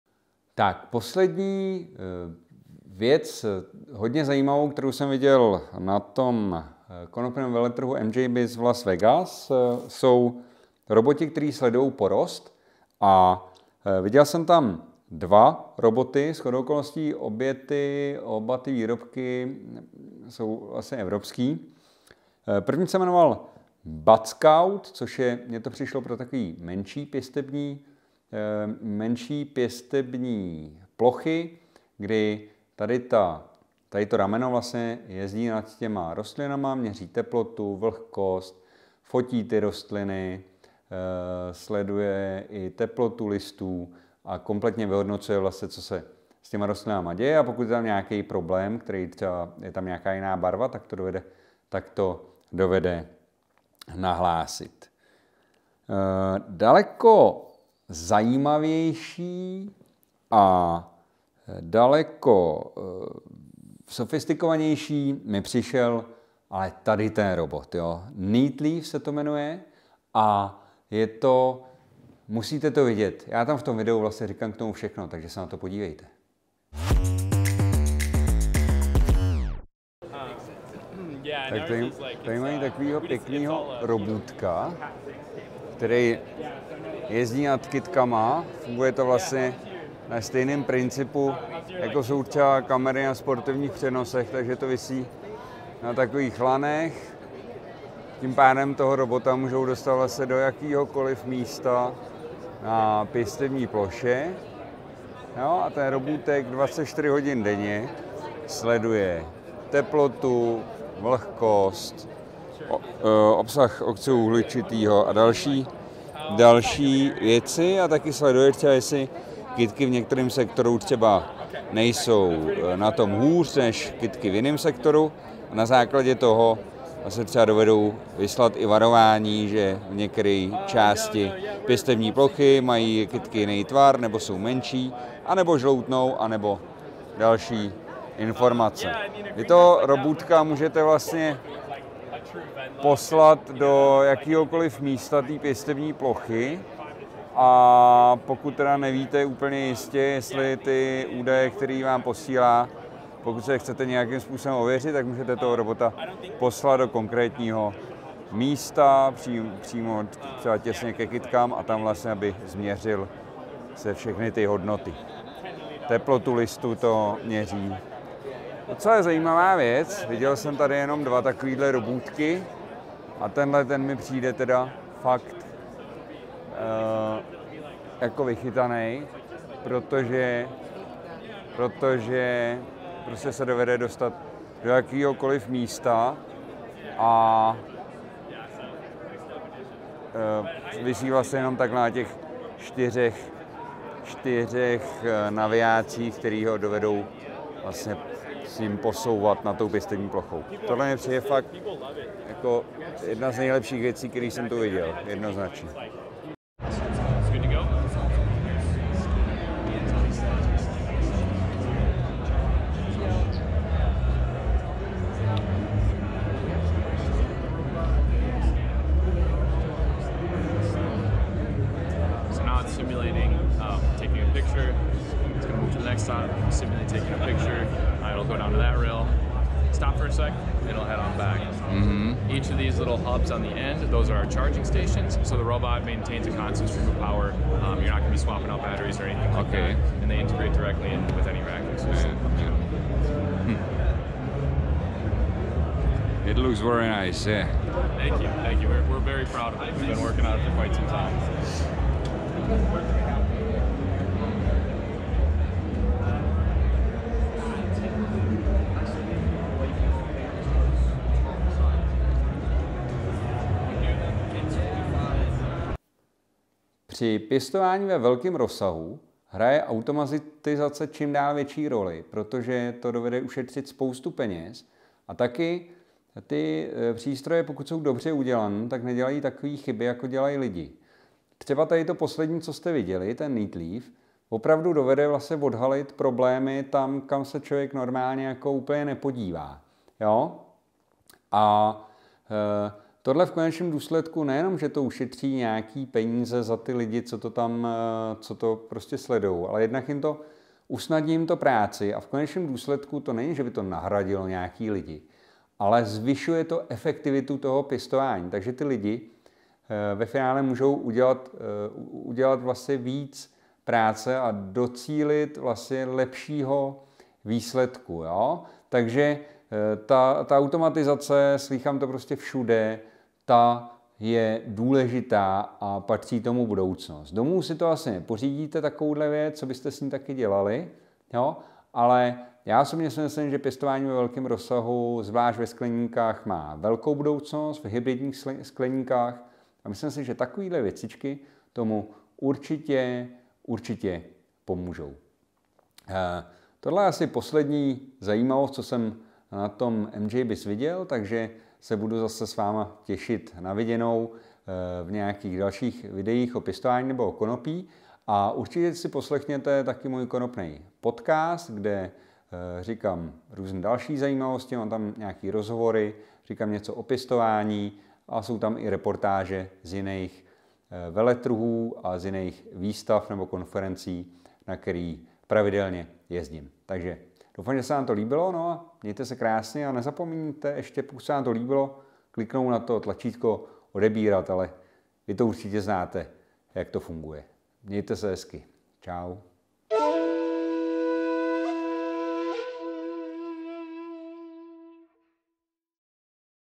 (5) Zajímavosti z konopného MJBiz v Las Vegas 2022
Automatizace pěstebního cyklu výrazně zefektivňuje celý proces. Mrkněte na robotické monitorovací systémy schopné včas odhalit problém a sledovat všechny potřebné parametry pěstebního cyklu.